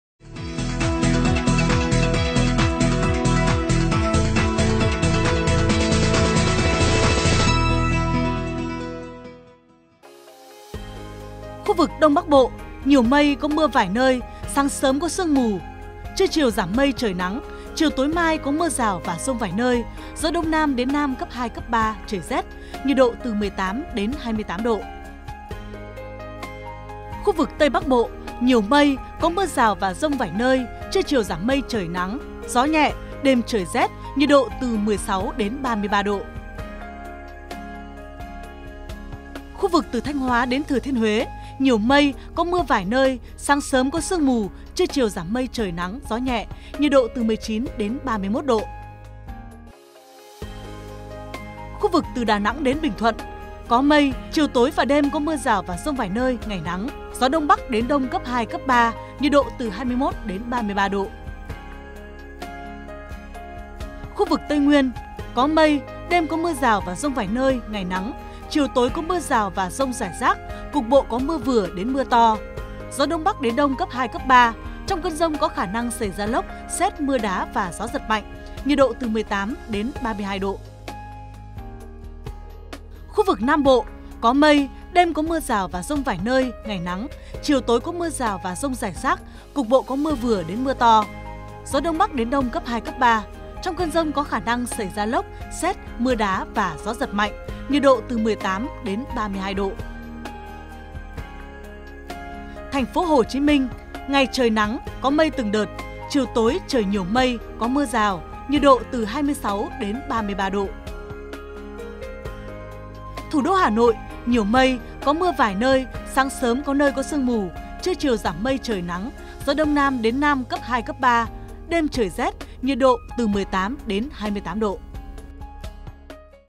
Dự báo thời tiết đêm 16/4 và sáng 17/4/2020 | Bắc Bộ đêm và sáng sớm có mưa rào rải rác
Bắc Bộ đêm và sáng sớm có mưa rào rải rác và có nơi có dông, sau có mưa rào vài nơi, riêng khu Tây Bắc ngày nắng. Trung Bộ chiều tối và đêm có mưa rào và dông vài nơi, ngày nắng. Tây Nguyên và Nam Bộ ngày nắng, đêm nay (16/4) có mưa rào và dông rải rác và lặp lại vào chiều tối mai (17/4).